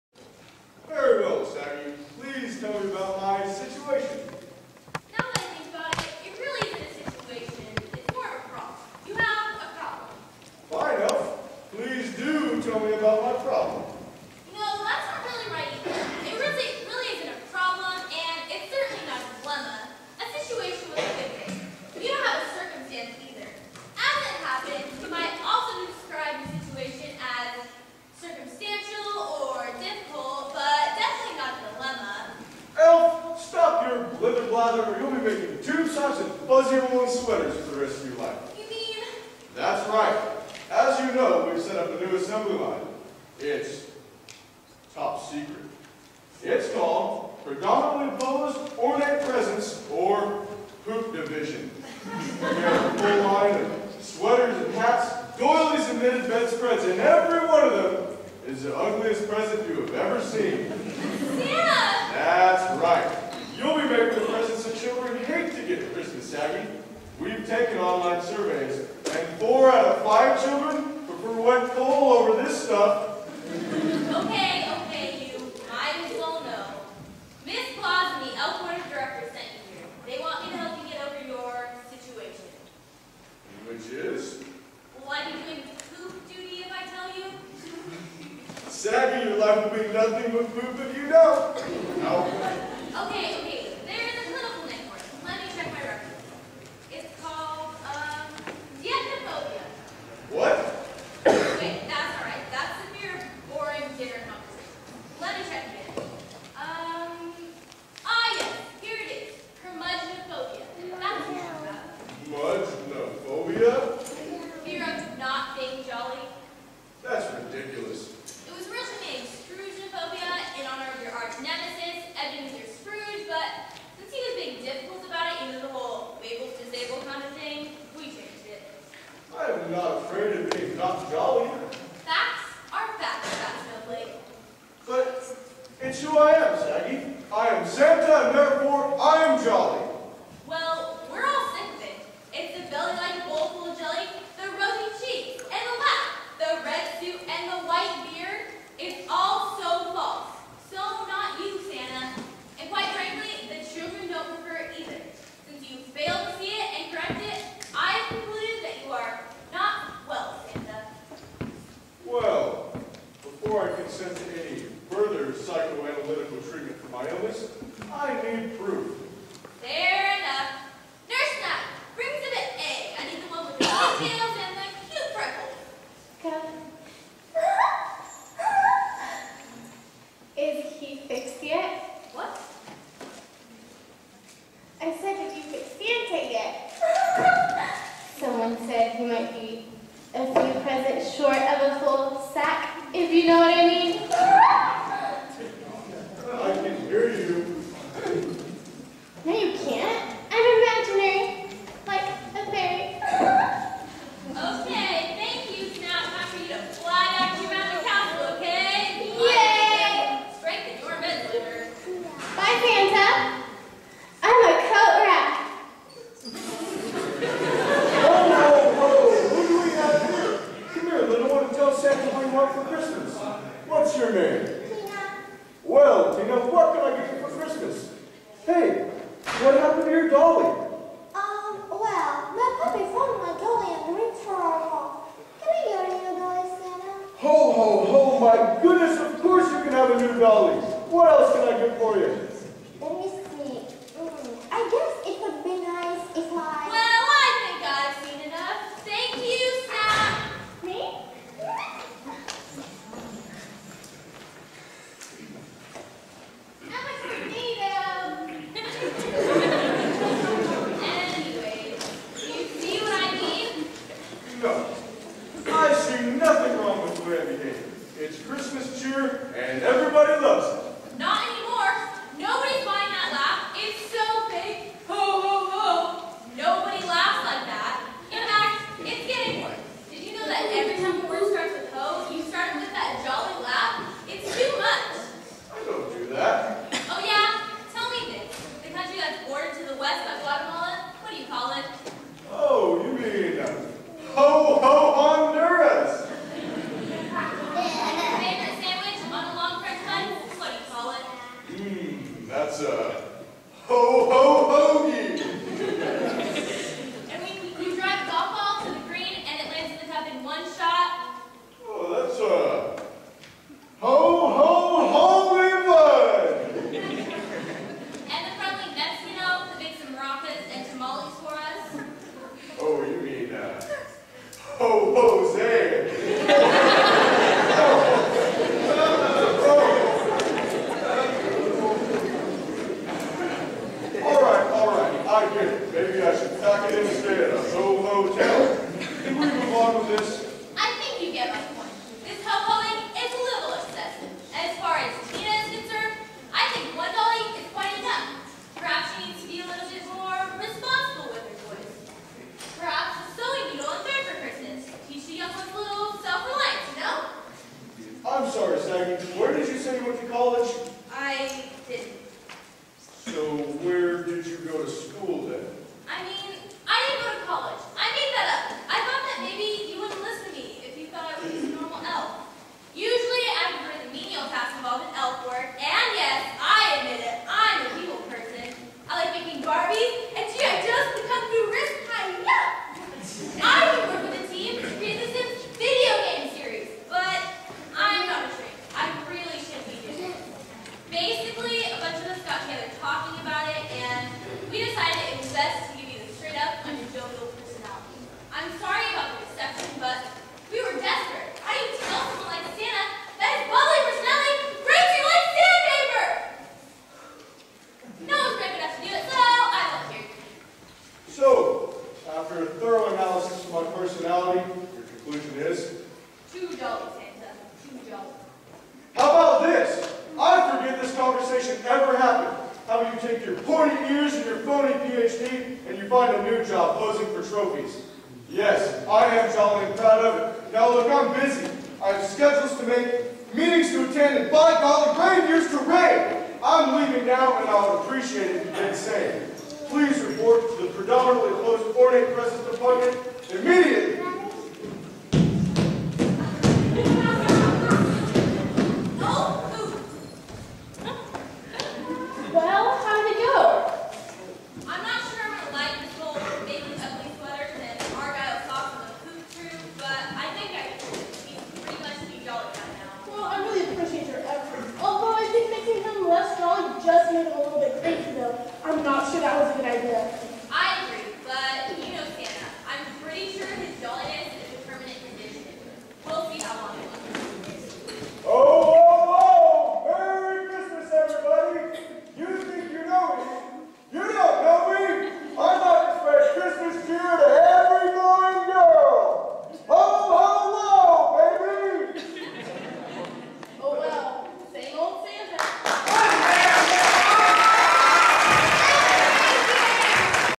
Santa's Crazy
The First Chronicle, entitled, 'Santa's Crazy'.  The North Pole psychiatrist, Saggie Drawers, PhD attempts to cure Santa of his 'jolliness".